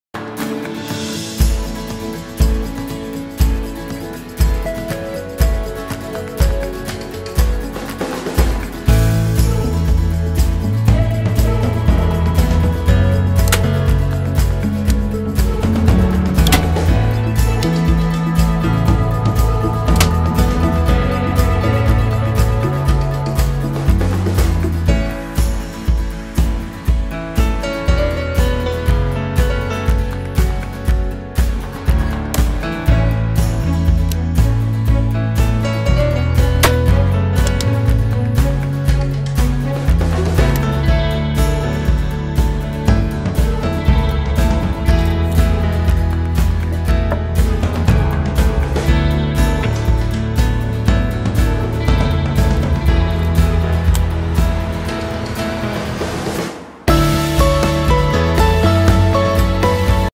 Rapid ALU953 Hand-Tacker 1001A-Tools.nl
Demonstratiefilm over het gebruik van de ALU953 Tacker van Rapid